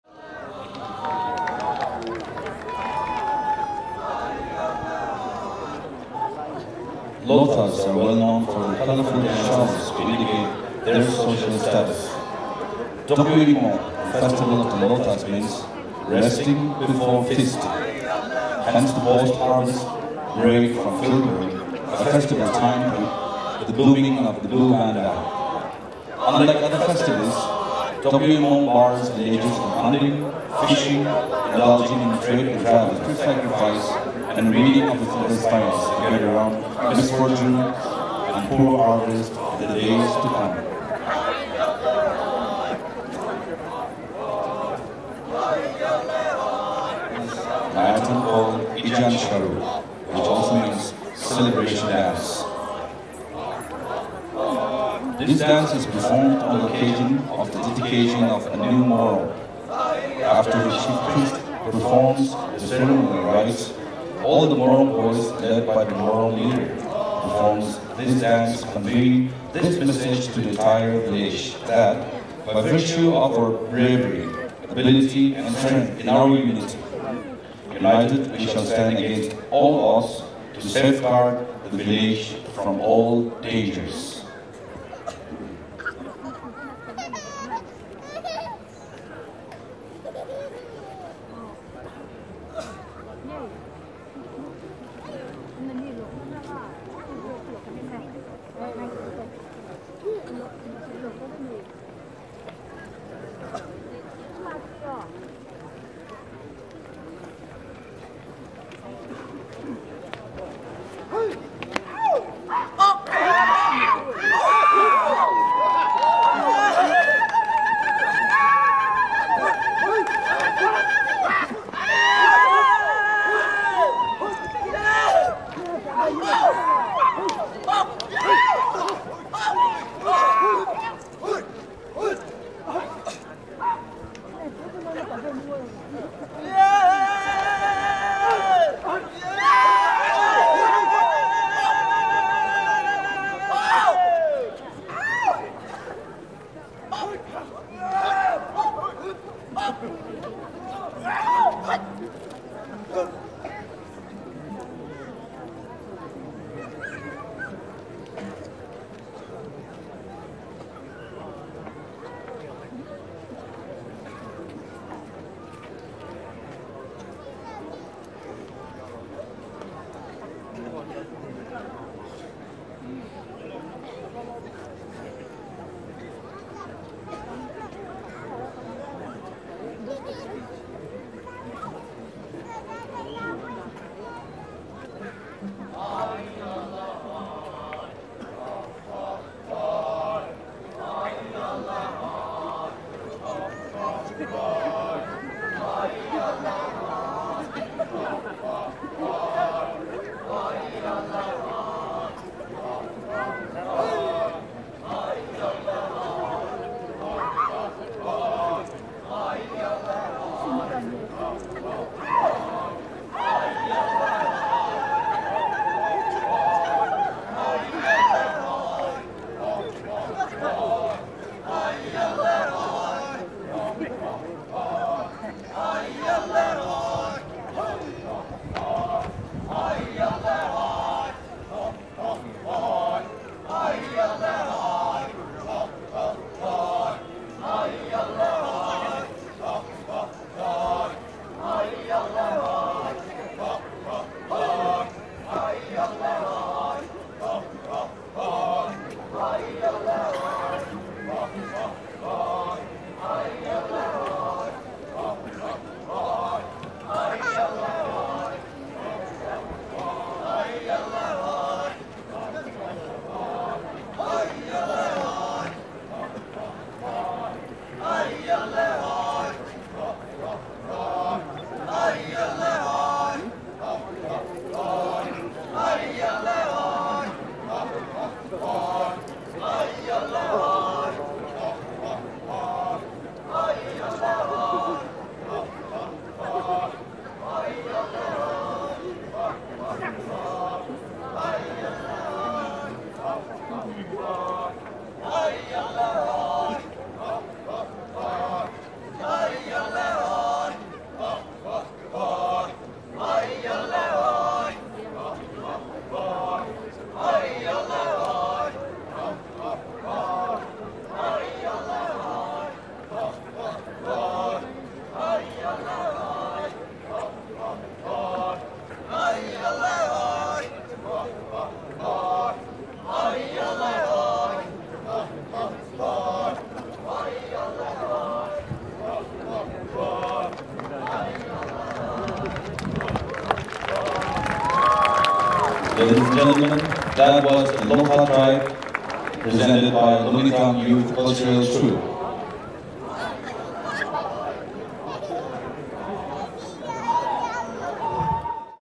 Lotha tribesmen performing a folk dance in Nagaland
A dance which is performed during the rise of new morung. The dance is performed during merry making and feasting.The lotha tribesmen not only perform traditional dance but also do some active sports.

The hornbill festival was created to bring together the various tribes of the State to display their culture and tradition in the form of dances, songs, sports, foods and many other colorful events. People from within India as well as foreign visitors experience and witness the colorful festival with great enthusiasm.

Source :- Wikipedia